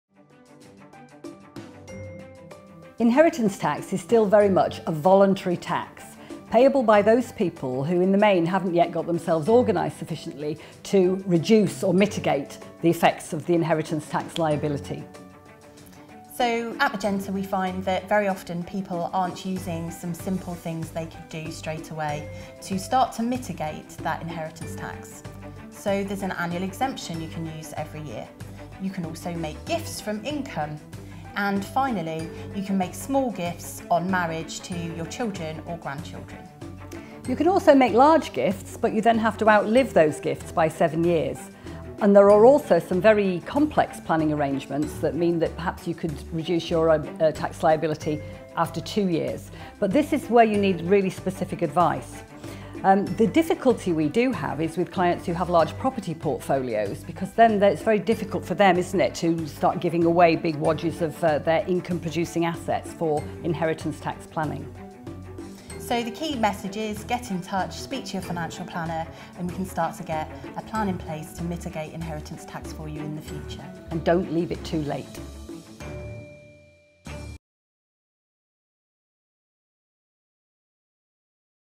Magenta on Inheritance Tax
Julie and Gretchen talk about Inheritance Tax